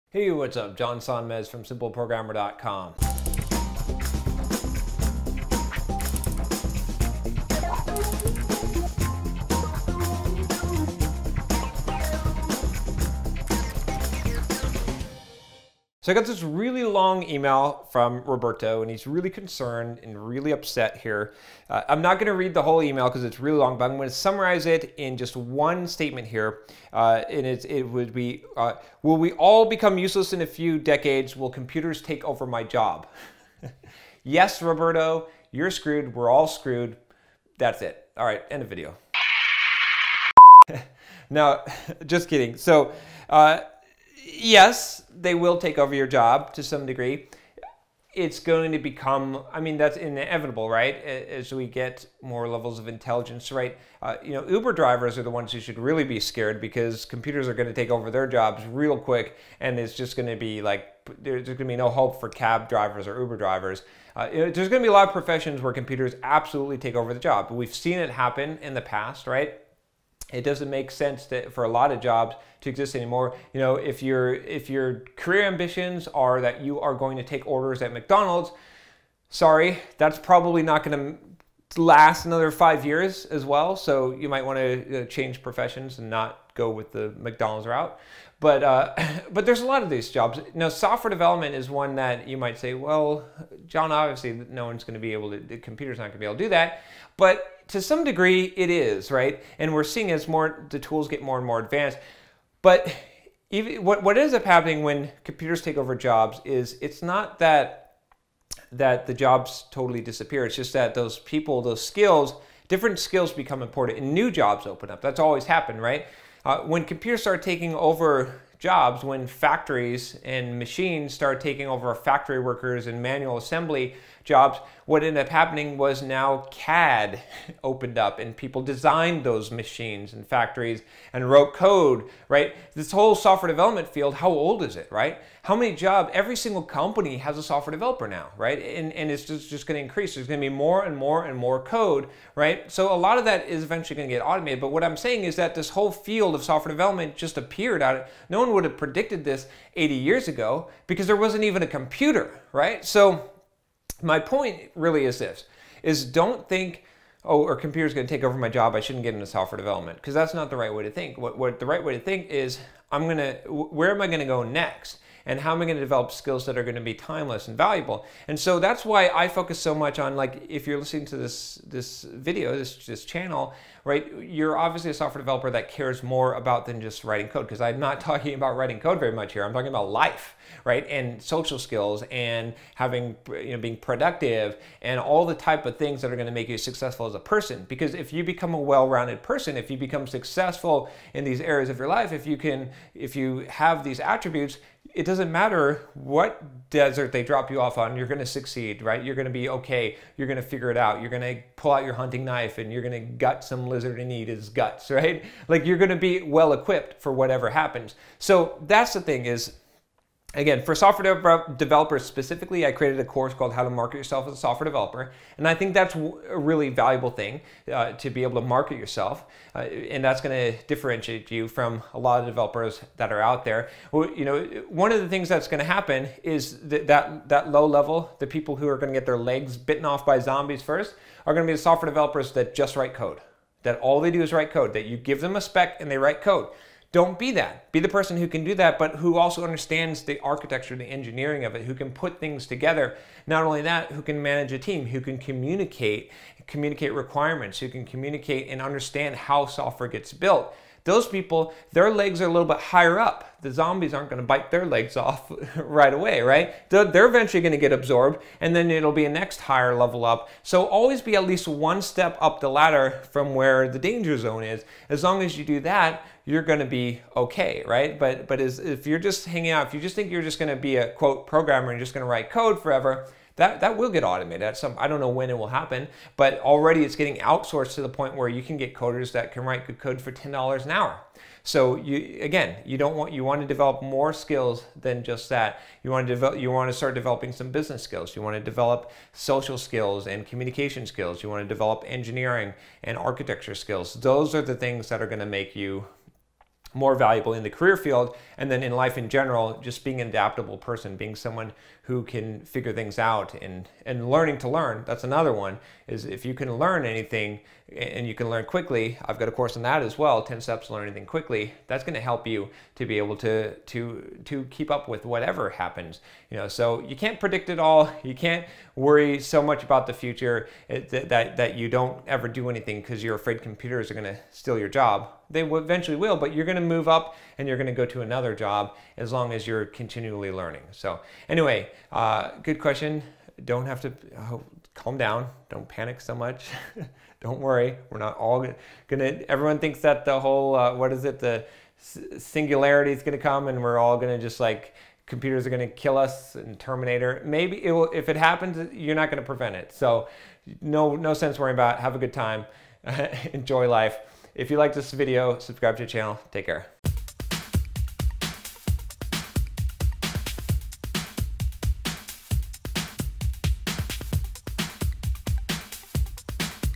Will We Become Useless When Computers Take Over Our Jobs? (AI)
Will We Become Useless When Computers Take Over Our Jobs?

Will all humans become useless on a future time when computers take over the jobs? Will computers replace us in a near future? Wel... The answer is YES! In some way...

The fact is that the automation process is inevitable. And with that automation process, comes a price that jobs are replaced by computers, robots and machines. What does it represent for you? It represents that you need to step up your game to avoid being eaten by the replacement zombies.

Are you interested in knowing how to survive in this near future? Watch this video and find out!